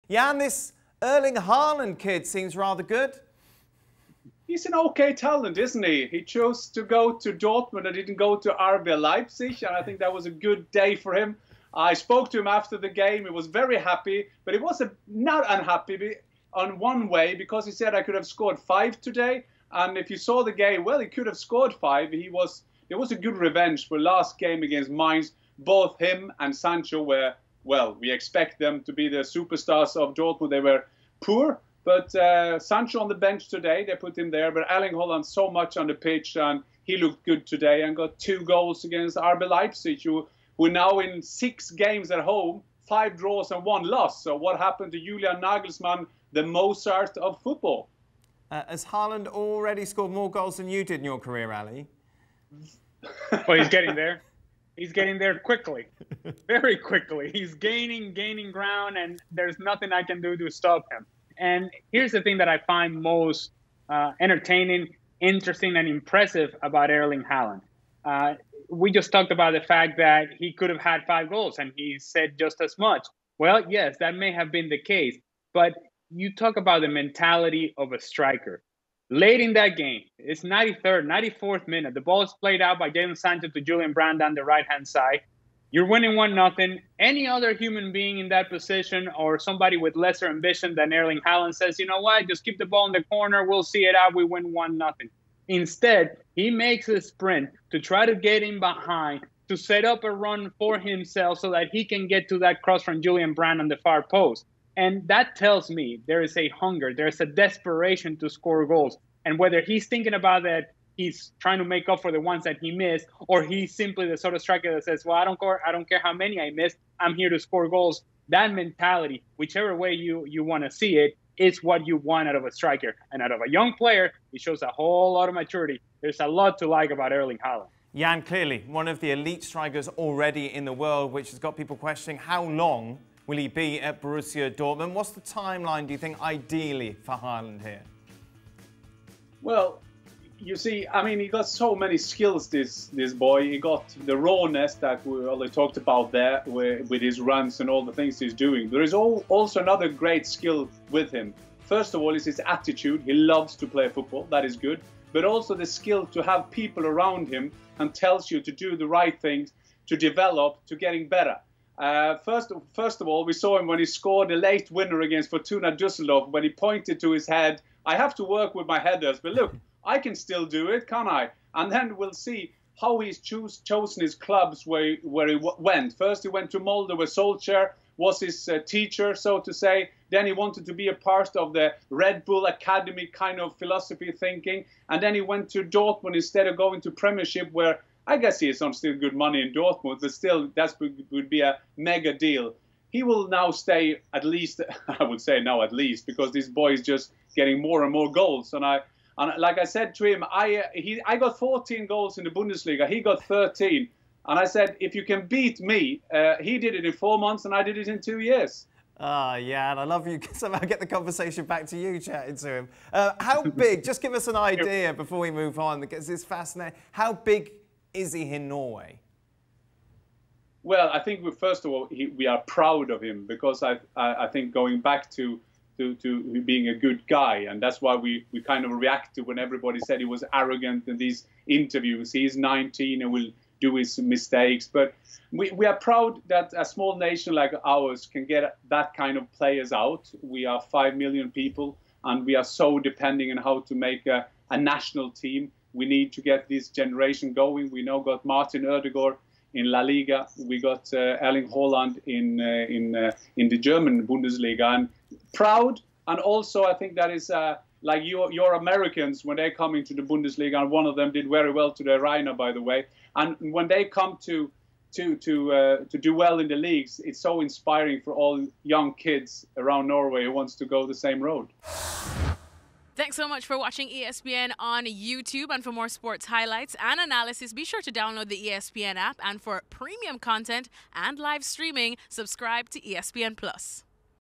Erling Haaland inspires Norway and makes us proud - Jan Aage Fjortoft | Bundesliga
Dan Thomas is joined on ESPN FC by Alejandro Moreno and Jan Aage Fjortoft to dissect Borussia Dortmund’s impressive 2-0 win over RB Leipzig thanks to two goals from young starlet Erling Haaland. Moreno outlines why Haaland is already an “elite striker” in world football, and compatriot Fjortoft tells us why he’s such an inspiration in their native Norway.